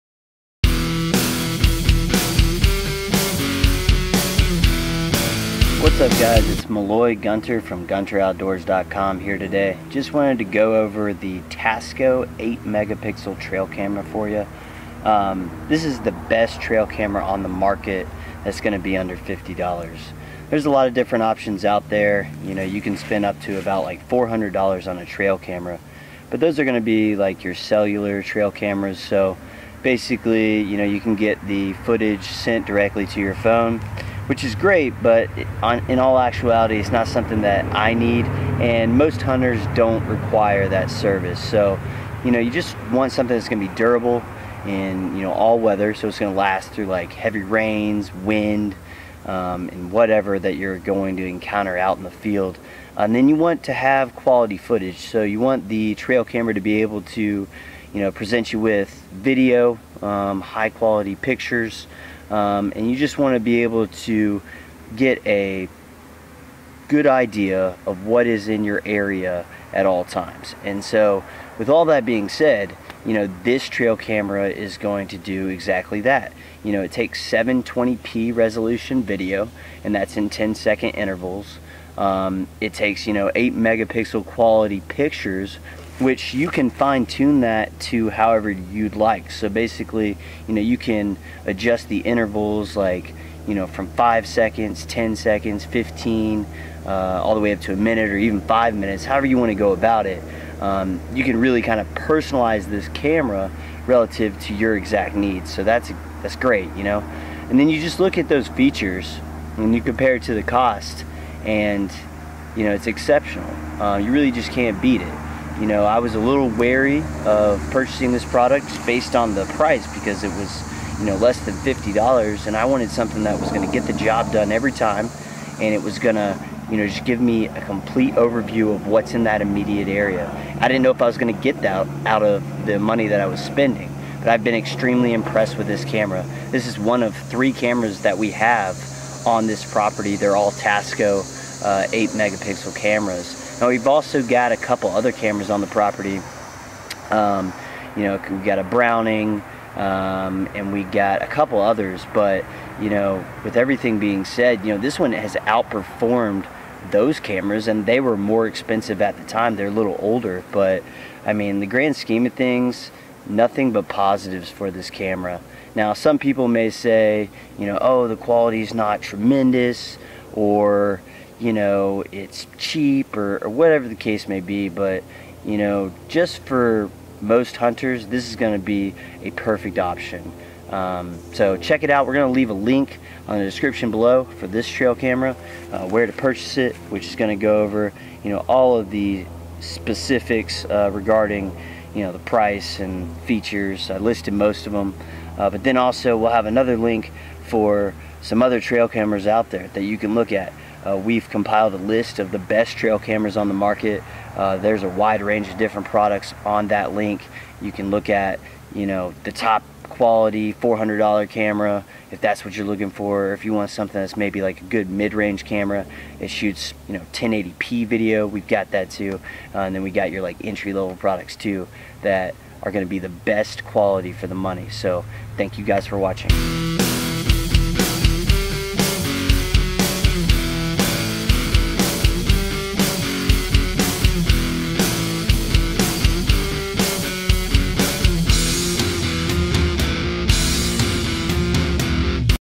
Tasco 8MP Trail Camera
The Tasco 8MP is the best trail camera available for under $50. This video will detail why it is so effective and cost efficient.